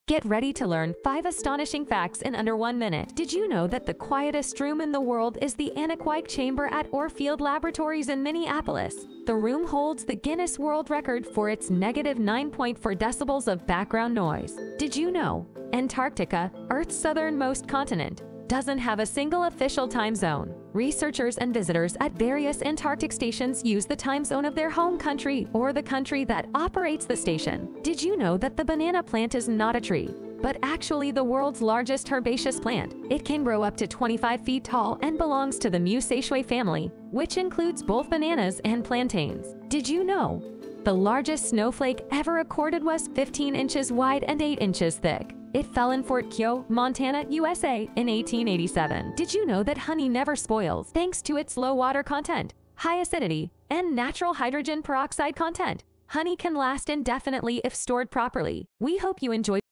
Did You Know? 5 Astonishing Facts in Under 1 Minute!
Description:
Prepare to be amazed as we uncover 5 astonishing facts in under 1 minute! From the world's quietest room to the fascinating banana plant, these facts will leave you wanting more.

Timestamps:
0:00 - Introduction
0:10 - Fact 1: World's Quietest Room
0:20 - Fact 2: No Universal Time Zone in Antarctica
0:30 - Fact 3: The Banana Plant
0:40 - Fact 4: The Largest Snowflake
0:50 - Fact 5: Honey Never Spoils
1:00 - Conclusion